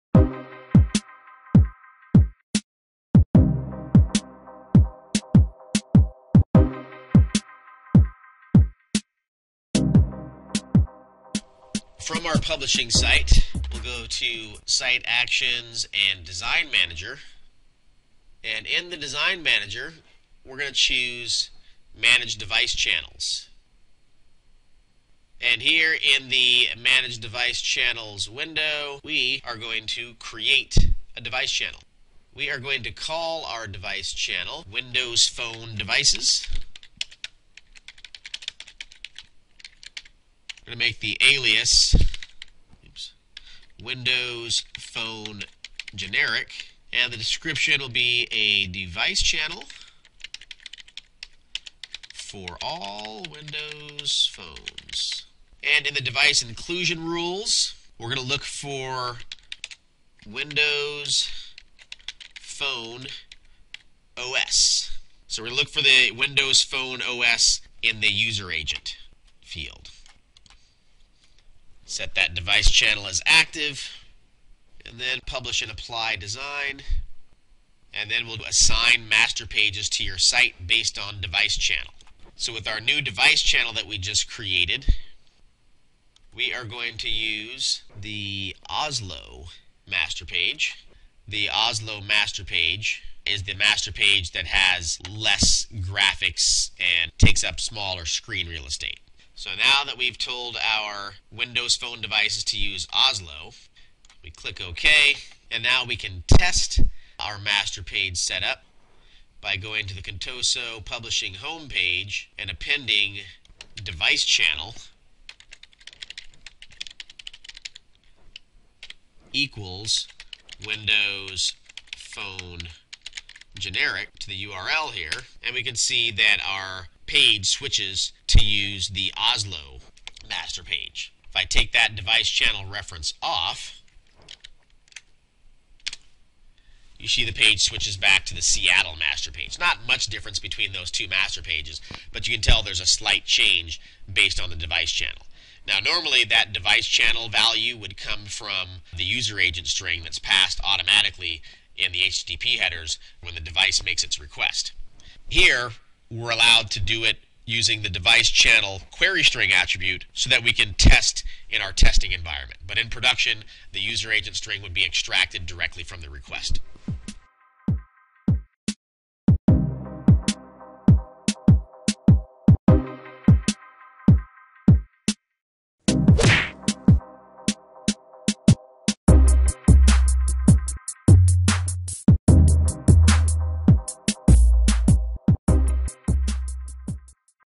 SharePoint 2013 - Create a Device Channel
In this Demo:
Create a Device Channel
Assign a Master Page to the Device Channel
Test the Device Channel using the DeviceChannel query string attribute
info @ ProDataMan. com